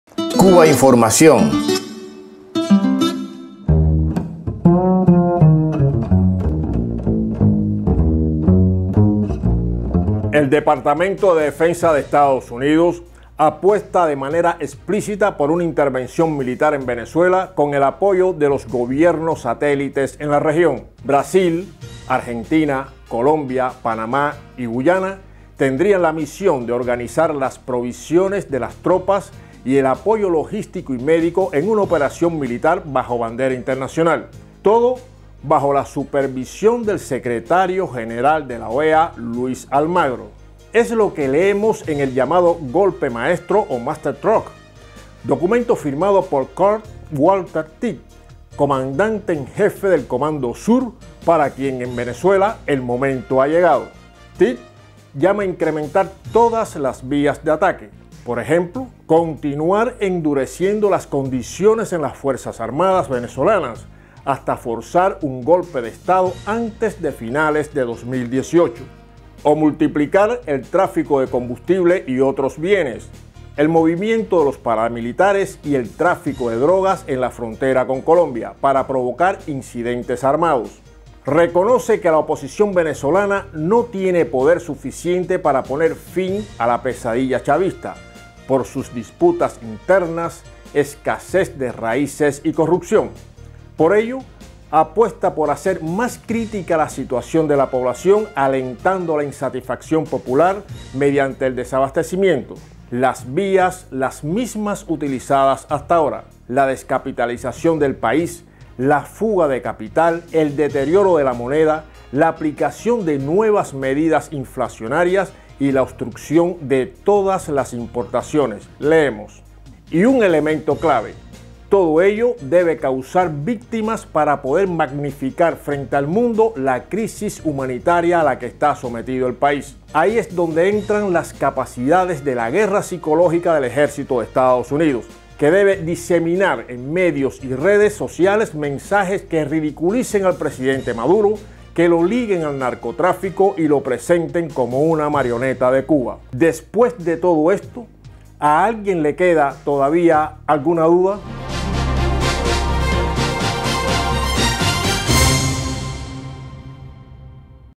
Masterstroke: ¿día y hora para la intervención de EEUU en Venezuela?
Cubainformación TV – Basado en un texto de Stella Calloni – Red Voltaire.- El Departamento de Defensa de EEUU apuesta de manera explícita por una intervención militar en Venezuela, con el apoyo de los gobiernos satélites en la región. Edición: Ana Gil. Fuente